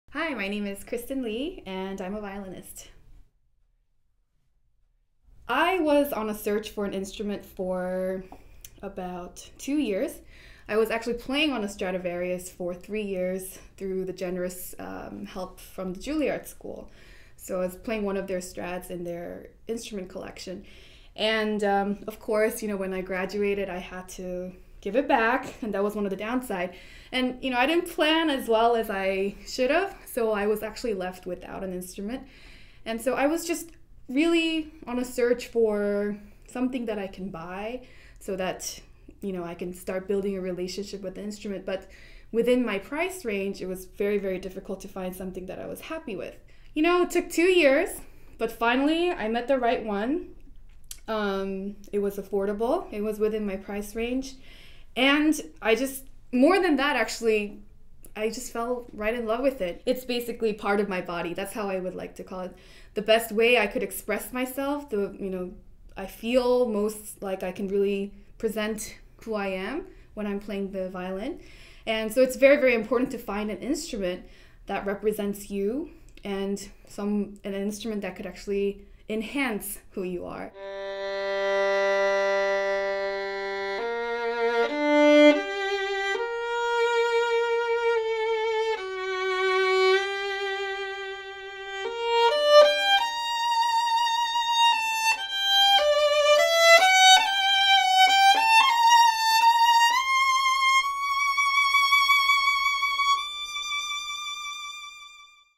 Kristin Lee talks to Tarisio
Violinist Kristin Lee is recognized as one of the most accomplished artists of her generation.  A violinist of remarkable versatility and impeccable technique. Ms. Lee was a recent winner of the Astral Artists '2010 National Auditions, and enjoys a vibrant career as soloist, recitalist and chamber musician.